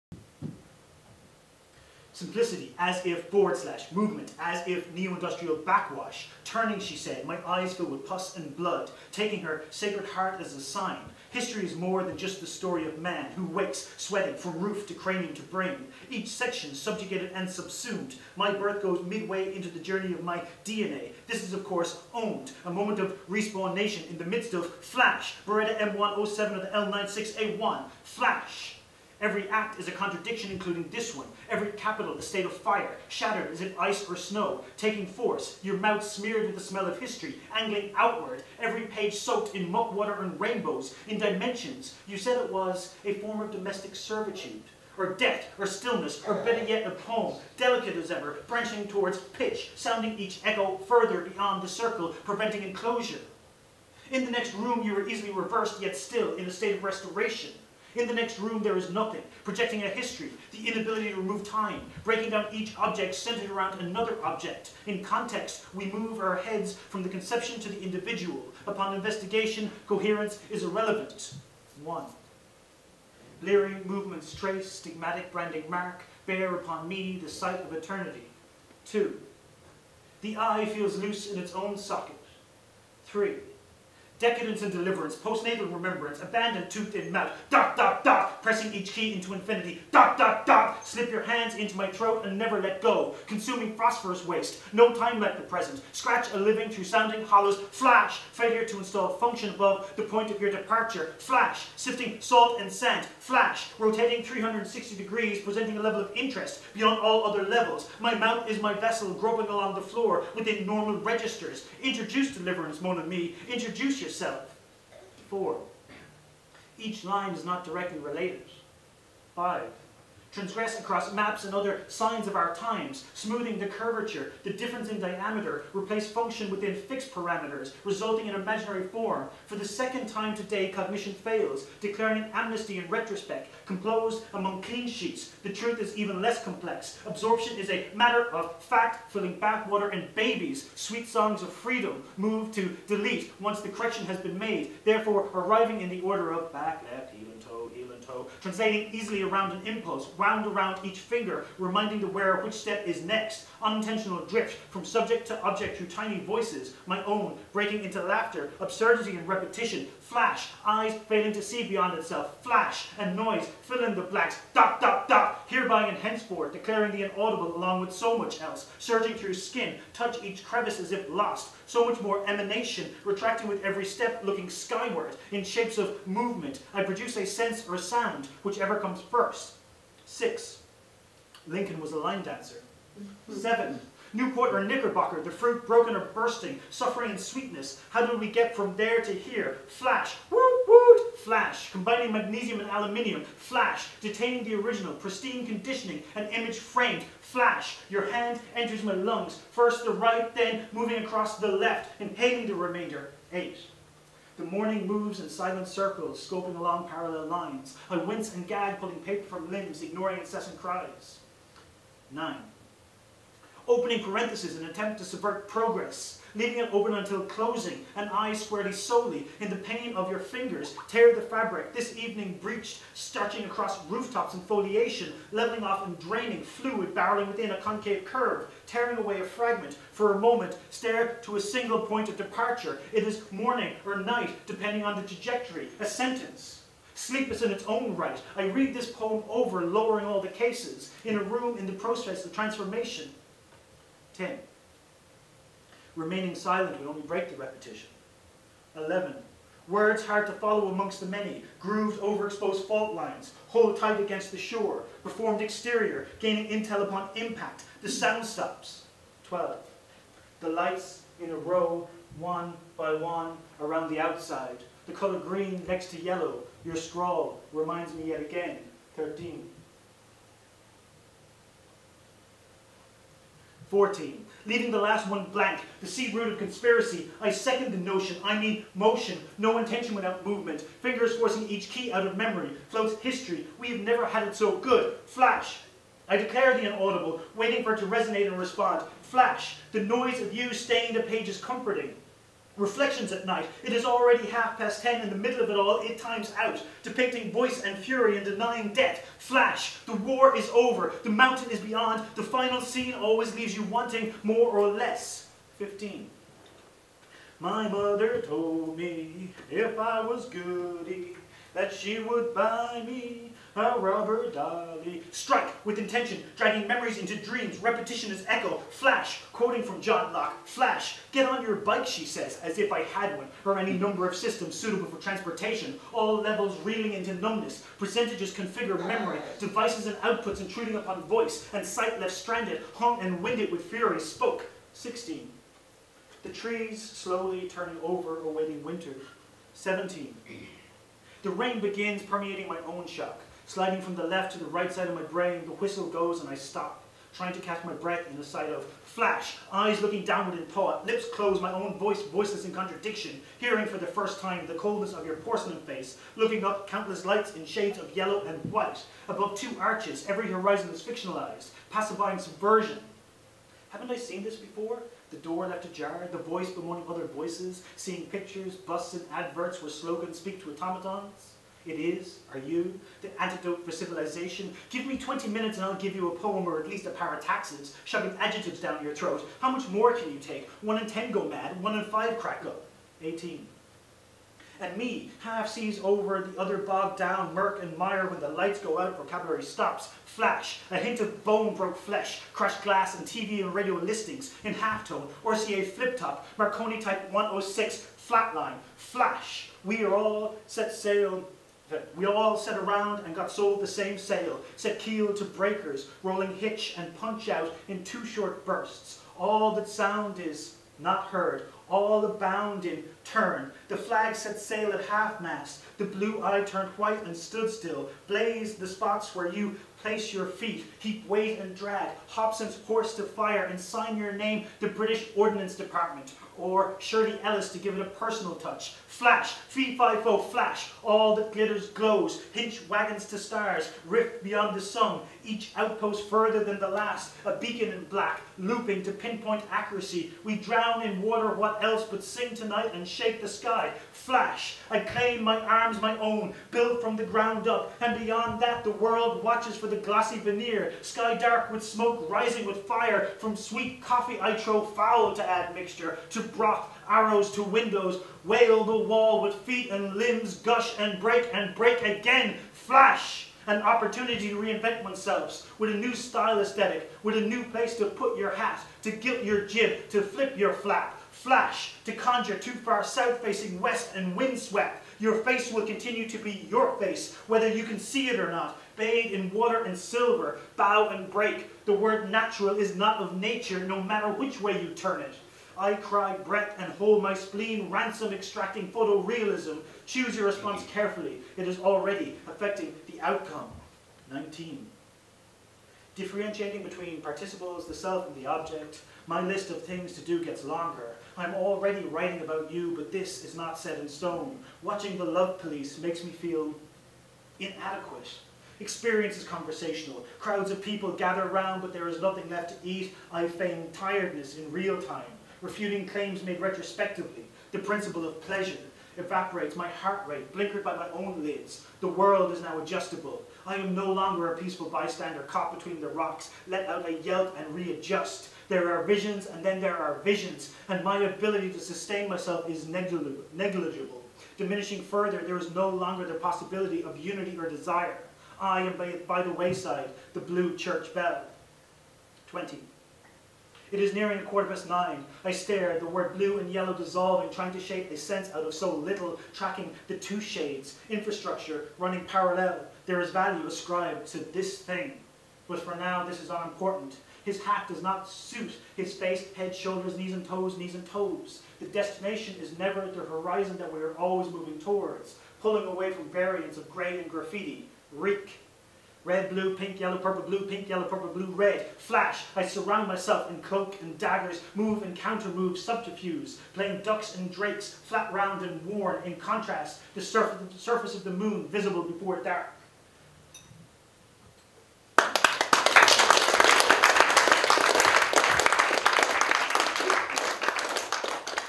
James Cummins - FLASH/BANG
James Cummins reads FLASH/BANG, at the Ó Bhéal Winter Warmer festival of poetry, Cork, Nov 16th 2013.